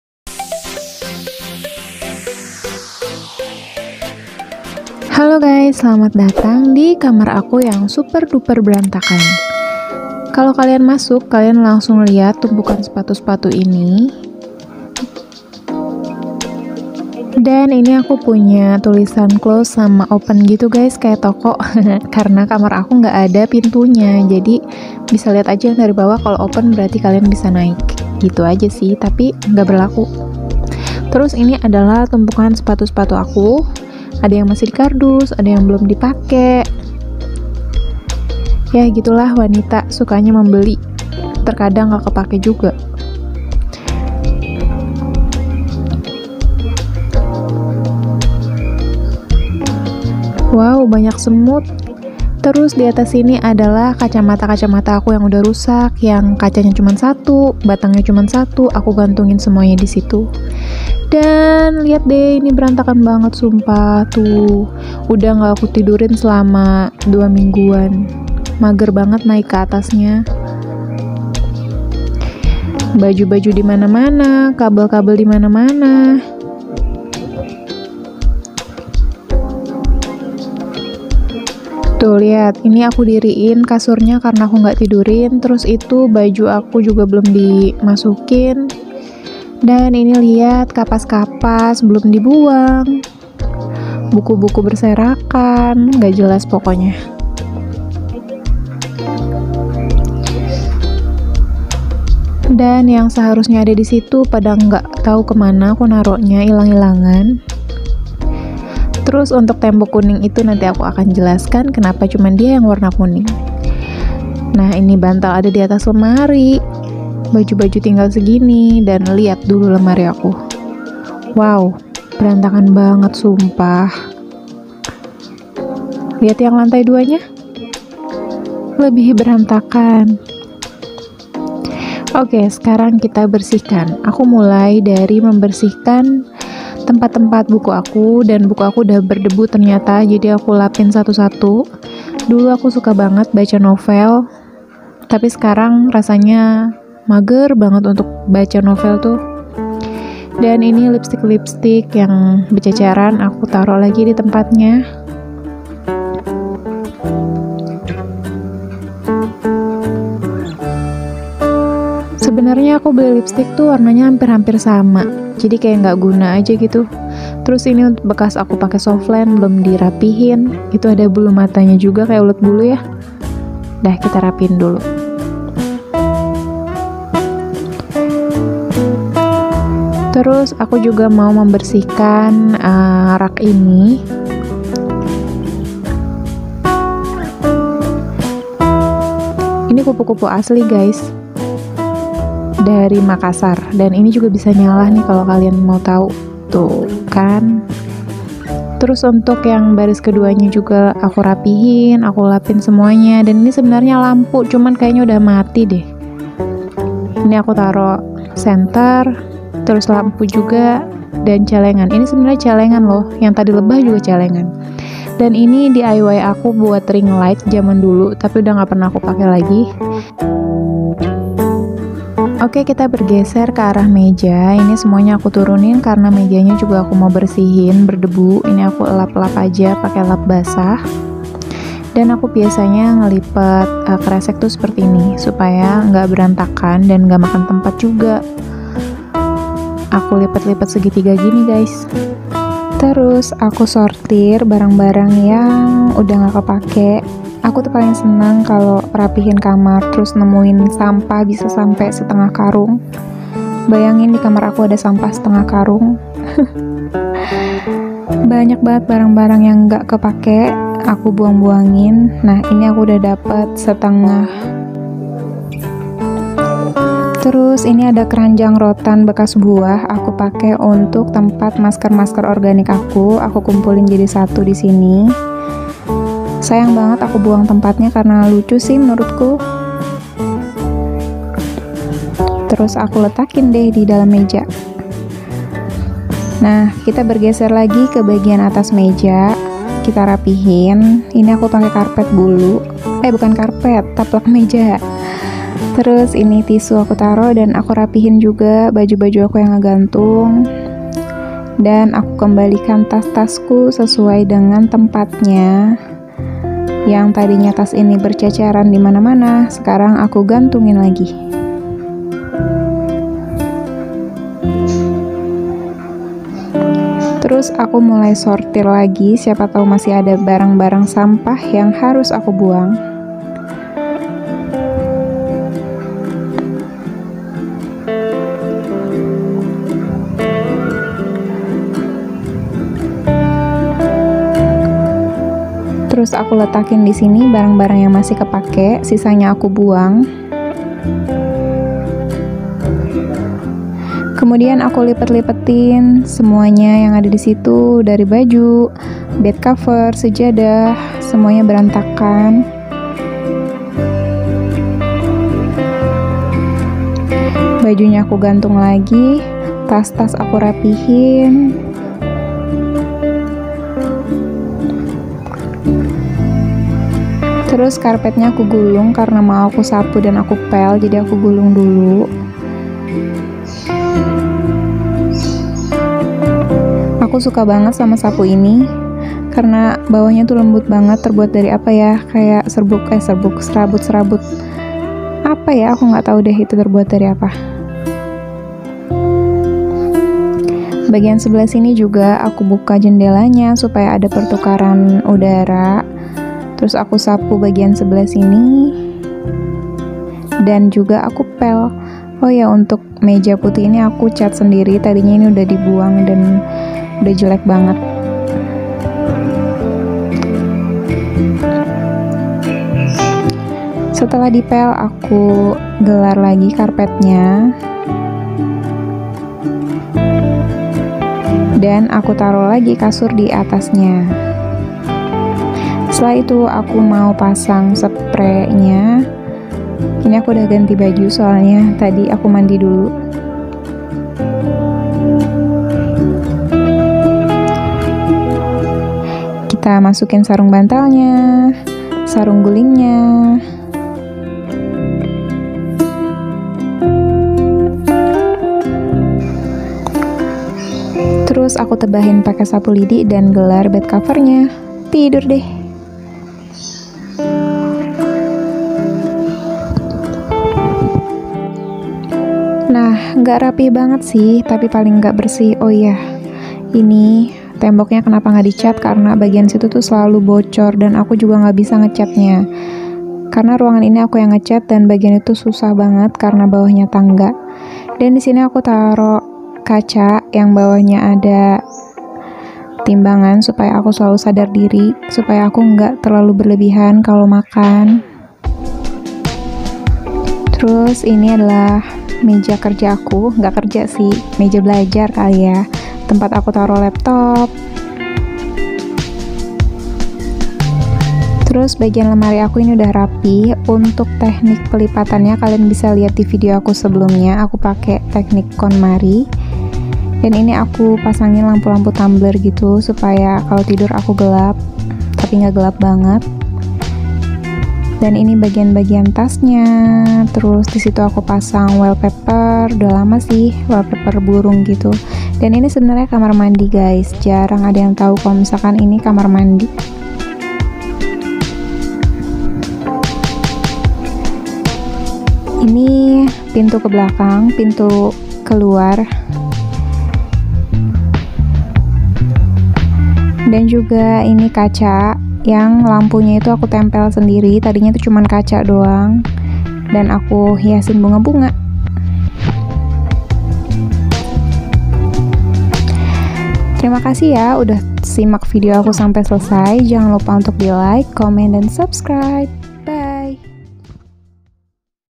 MEMBERSIHKAN KAMAR KECIL | CLEANING MY BEDROOM | CLEANING MOTIVATION INDONESIA | CLEAN WITH ME
Assalamualaikum wr.wb
Halo teman teman, di video kali ini aku ingin berbagi bagaimana ketika aku membershikan dan merapikan kamar tidur.
Semoga video ini memotivasi kalian juga yaa untuk ikut bersih bersih.